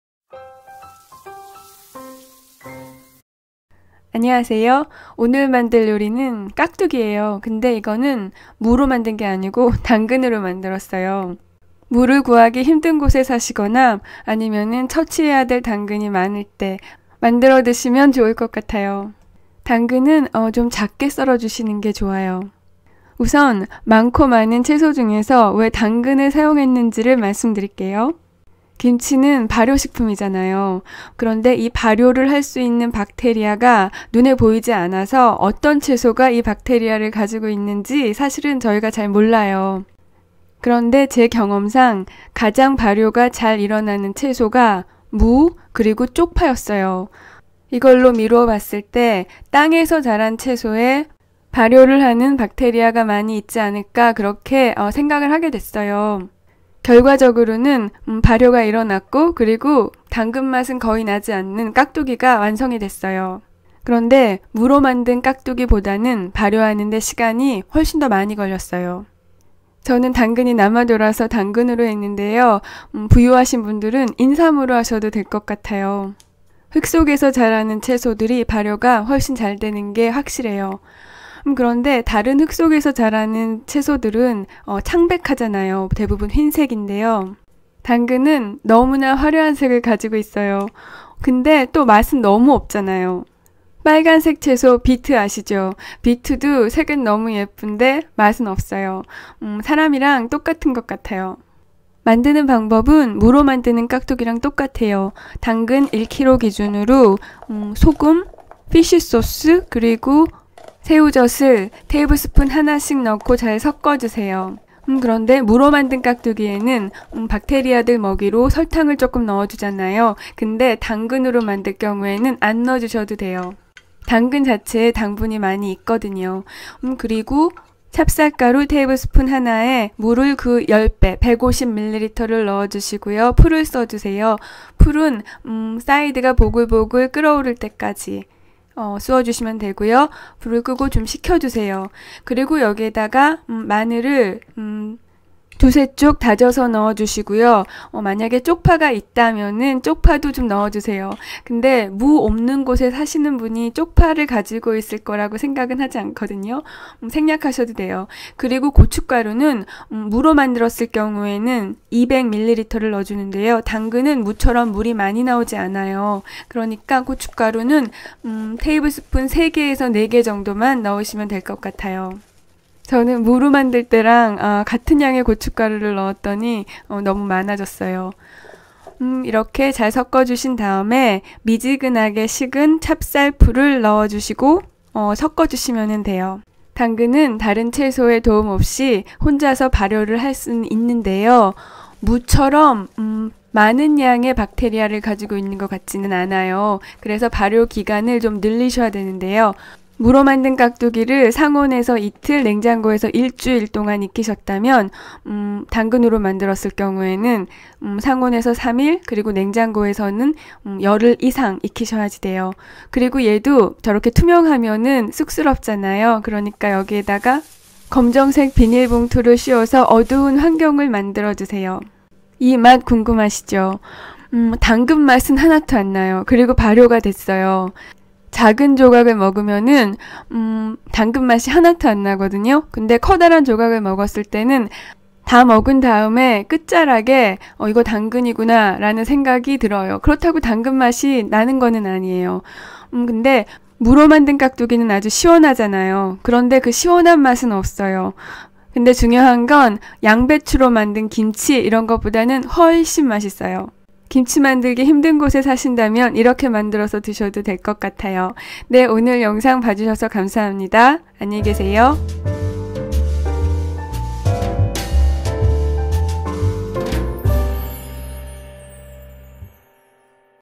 무가 아닌 '이것'으로 만든 깍두기
재료:
천일염 1 Tbsp
새우젓 1 Tbsp
피쉬소스 1 Tbsp
찹쌀가루 1 Tbsp
물 150ml
고추가루 3~4 Tbsp
다진 마늘 2~3 쪽
당근 1kg